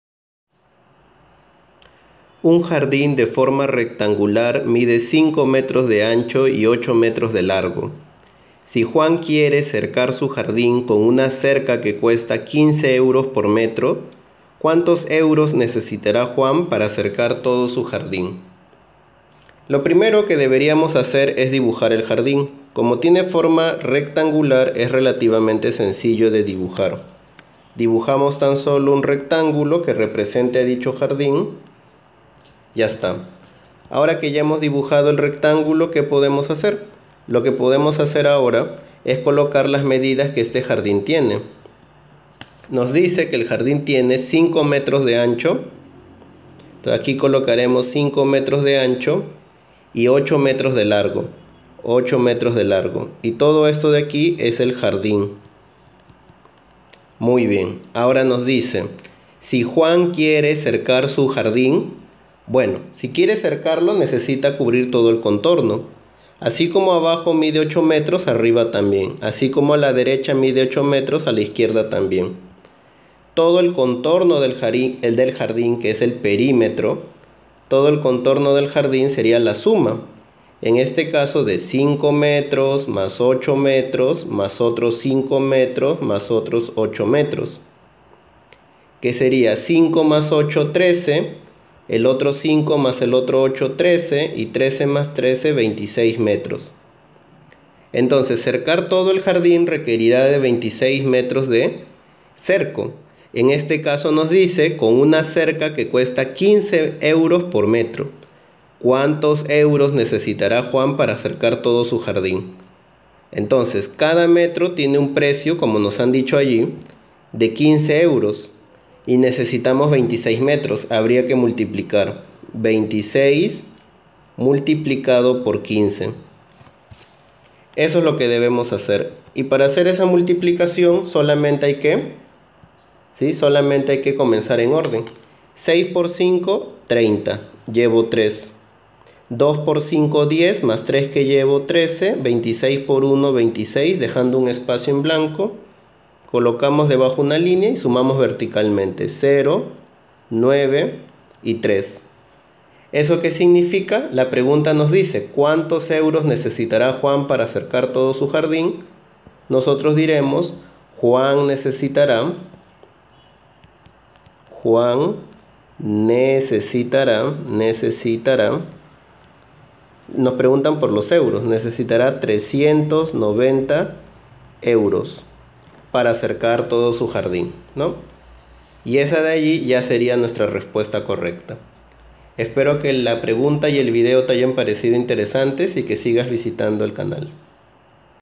Problema resuelto sobre perímetro y multiplicación
Un jardín de forma rectangular mide 5 metros de ancho y 8 metros de largo. Si Juan quiere cerca su jardín, con un cerca que cuesta 15 euros por metro. ¿Cuántos euros necesitará Juan para cerca todo su jardín?

Espero que el video te resulte útil y sigas visitando el canal.